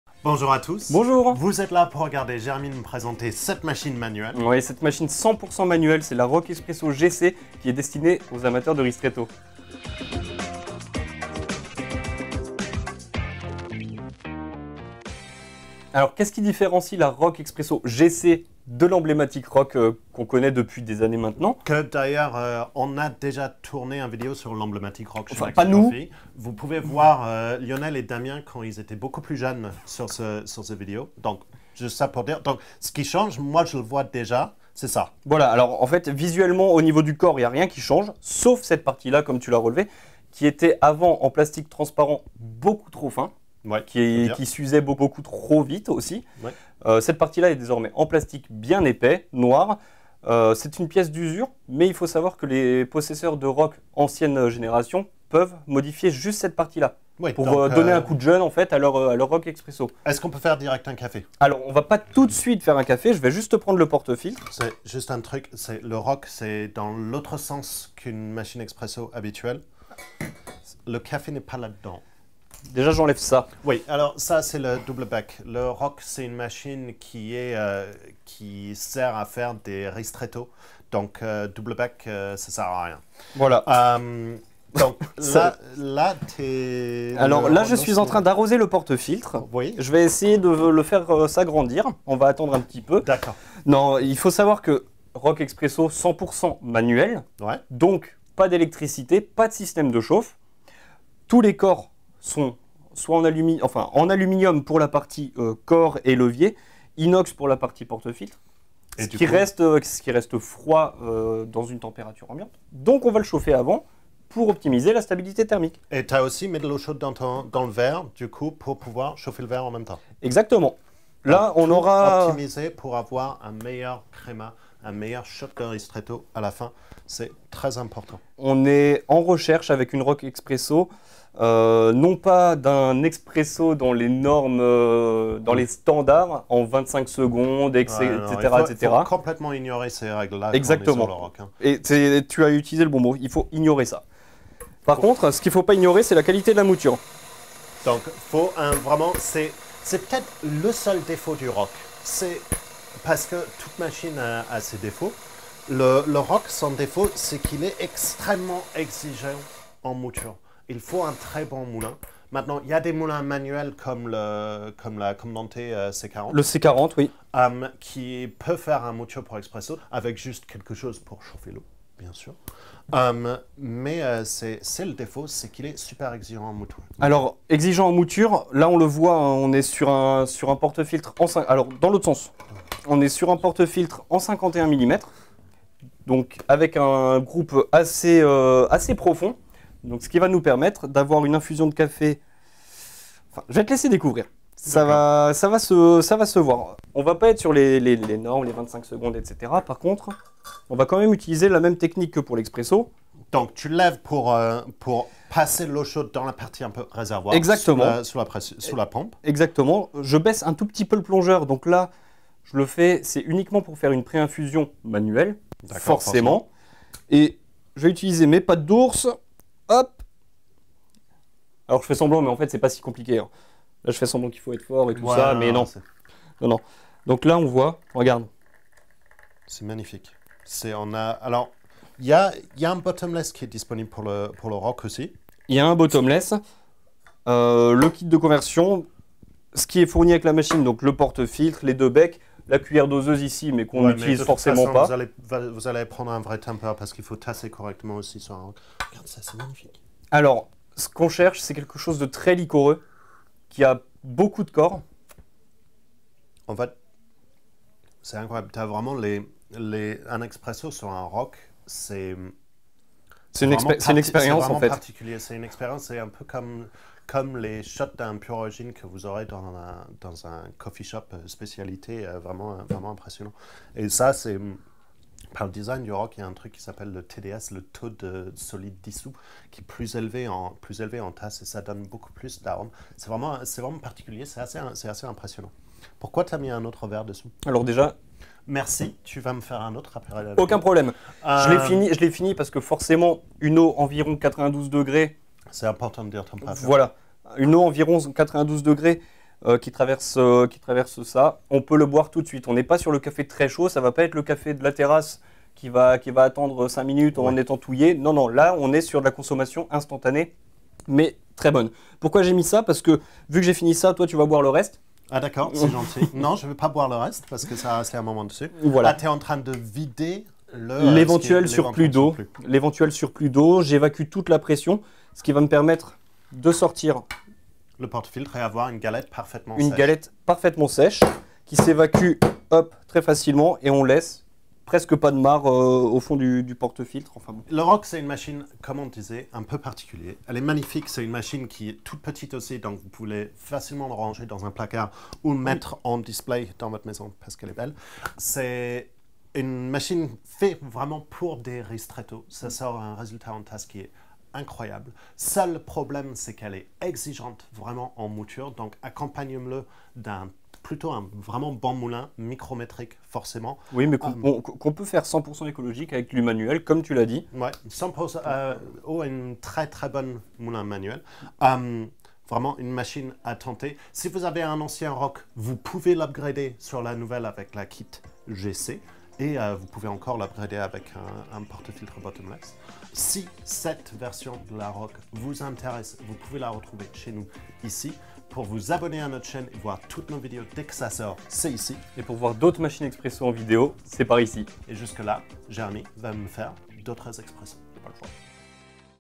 ROK ESPRESSO GC | Machine expresso manuelle | Le Test MaxiCoffee
Nos experts vous présentent la Rok Espresso GC une machine espresso manuelle dédiée aux amateurs d'expresso ou de ristretto avec une vraie puissance aromatique et un corps à faire pâlir les meilleurs bars italiens. Sans capsule ni dosette, il suffit d'avoir la bonne mouture de café frais et un peu d'huile de coude pour activer les deux bras de pression.

Ce modèle Rok Espresso GC est une version améliorée de la légendaire Rok Espresso. Le réservoir d'eau ainsi que le plongeur sont désormais dans un plastique alimentaire dur pour plus de sécurité.
Le plastique transparent qui composait la Rok Espresso avait parfois tendance à s'user rapidement, c'est désormais de l'histoire ancienne grâce aux corrections apportées par Rok.

Avec la ROK Espresso , préparez un véritable expresso sans capsule et sans électricité (il faut tout de même chauffer l'eau avant). Ecolo, pratique et fonctionnelle !

Vous redécouvrirez le goût de l' expresso , bien meilleur et authentique qu'un café en filtre ...et moins cher.

Son plus? Son fonctionnement totalement silencieux, qui vous permettra de préparer et déguster votre café à tout moment de la journée.

La ROK Espresso fonctionne de la même façon qu'un percolateur traditionnel. Vous n'avez qu'à remplir le porte filtre  (diamètre du porte-filtre gradué : 49 mm pour 1 tasse, 51 mm pour 2 tasses ) avec le café de votre choix. Versez ensuite de l'eau chaude sous pression, pression que vous obtenez non pas grâce à un moteur électrique comme pour un percolateur , mais par la pression manuelle des bras de la machine . Elle excelle dans la réalisation de ristretto (3/4 cl), là est vraiment son gros point fort.

Cette manipulation manuelle permet d'obtenir un café excellent, digne des meilleurs percolateurs. Après cela, vous n'avez plus qu'à déguster votre café fort en arômes ...Bonne dégustation !

ATTENTION Un moulin est obligatoire pour avoir un bon résultat avec cette machine ROK Espreso. Consultez notre rubrique Moulin à café .